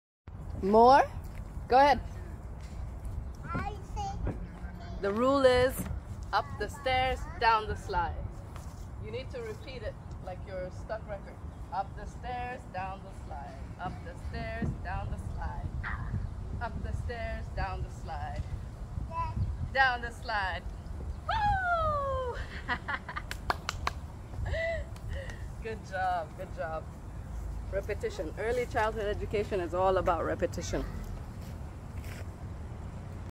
-::{Instruct - Play - Repeat}::-
Play is essential to children's development; it is their only job in life at the moment. Your job as parents is to encourage it, but also coach (with LOTS of encouragement) along the way. Repetition in giving instructions is fundamental, especially for rules that fall into social norms, or protect them from harm. They need to understand from the very beginning what is OK and what is not. Play is your friend, parents. Play builds on everything from logic, emotions, fine motor skills, and social norms/adaptability (taking turns, different playgrounds etc).

So go, play. Learn together. Above have fun!

You were a kid once, try and think back to your bubbles of childhood excitement. Let those be your emotional guides. Be goofy - your kids dgaf if you're a teacher, CEO or driver; you're just mom or dad.

Happy Leaning.